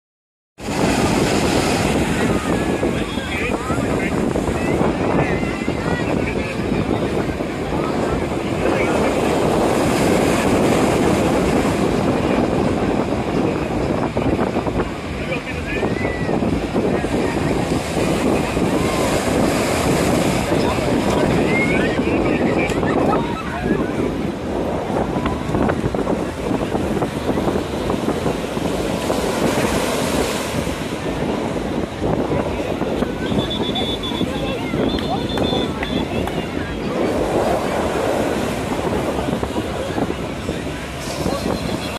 Baga beach 🏖️ North Goa
Baga is a famous beach destination in North Goa that attracts thousands of tourists and watersports lovers.